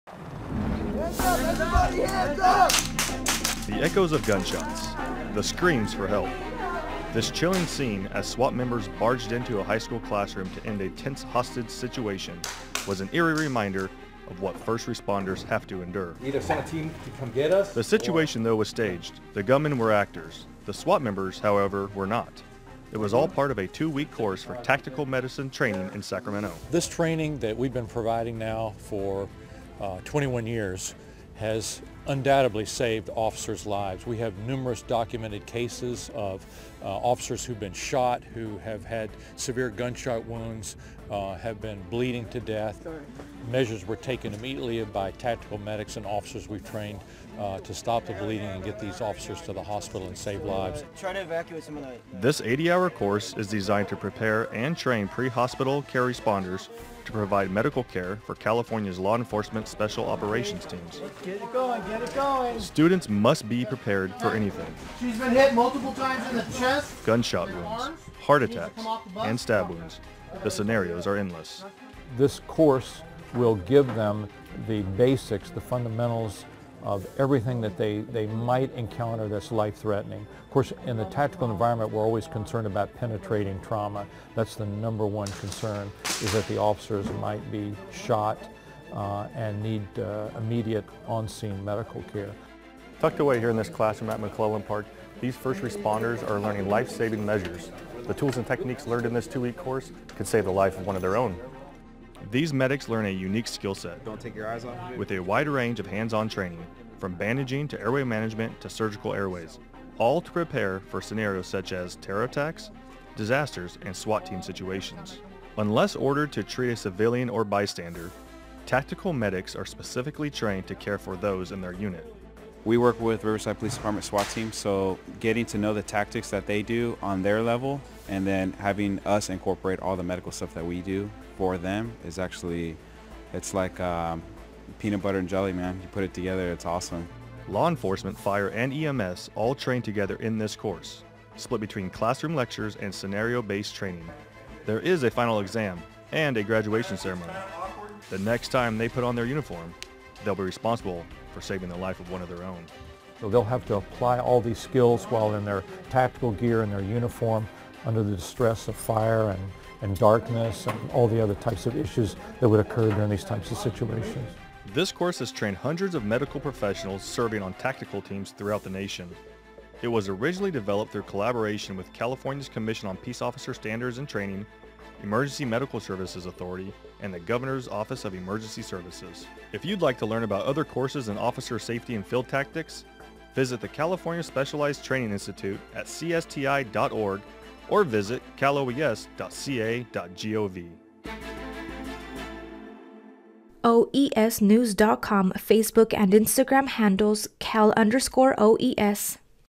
Emergency Medical Responders Get Tactical Training
Emergency medical first responders are trained on what to expect and how to respond in a potentially violent situation.